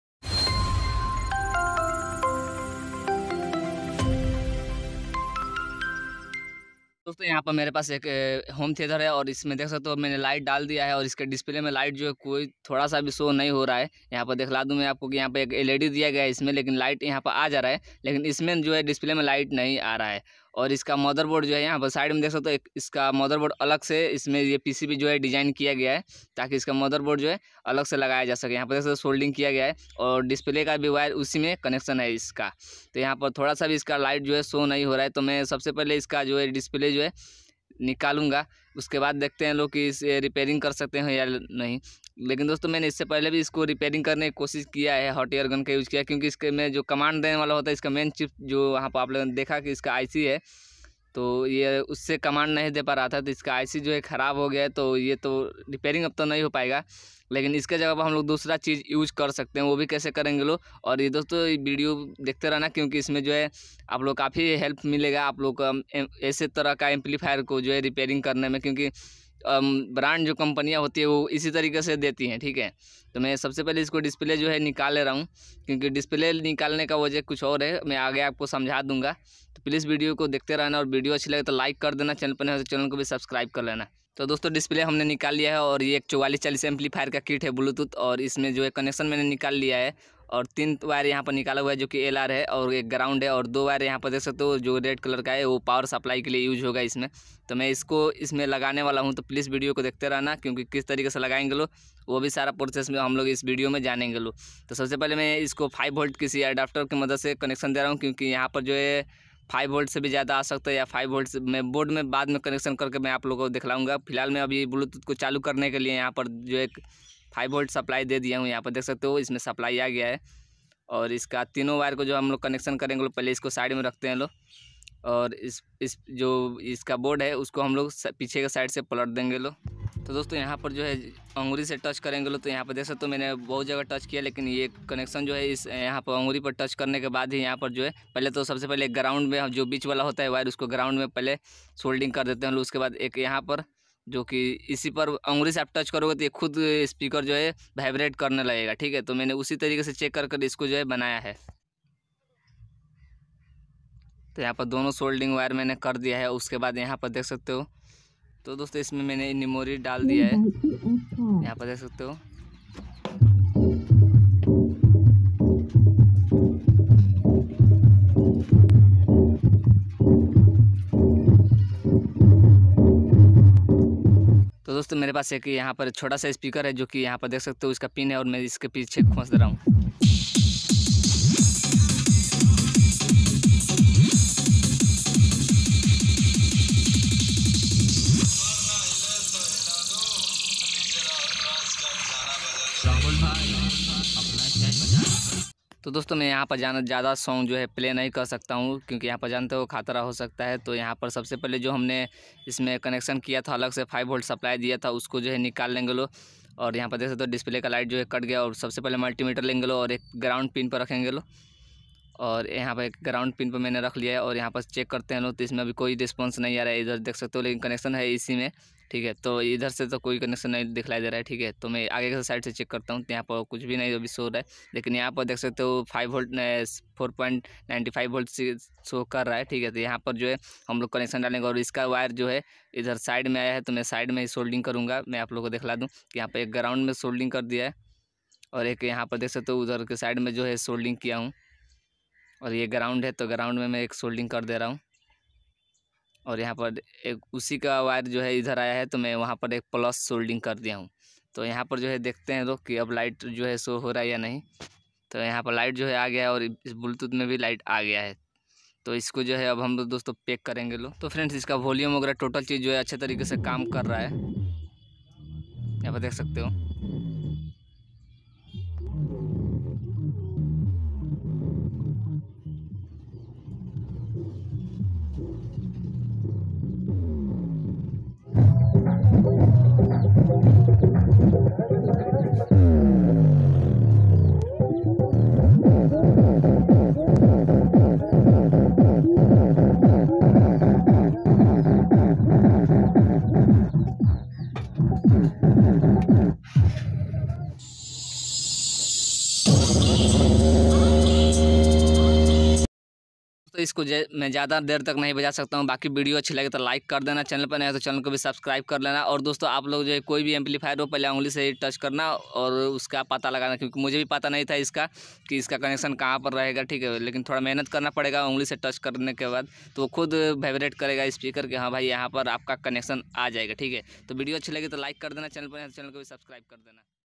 Home Theatre Bluetooth display problems solve 100% || How To Repair Bluetooth display ठीक कैसे करें
Home Theatre Bluetooth display problems solve 100% || How To Repair Bluetooth display ठीक कैसे करें Bluetooth_HomeTheatre_2.1/4.1/5.1

hello friends is video mein main bataya ki aap log kaise Bluetooth kit ko change kar sakte ho vah bhi subwoofer ka 4.1 ho ya 2.1 Ho Kisi mein bhi aap log change kar sakte ho Baki video acchi Lage to like karna channel per channel ko bhi subscribe kar lena aur kaise change karna is video mein bataya hun to please video ko Shuru Se last Tak Jarur dekhna

Bluetooth kit
 Bluetooth 5voltage
Bluetooth supply
 Bluetooth
Bluetooth
 Bluetooth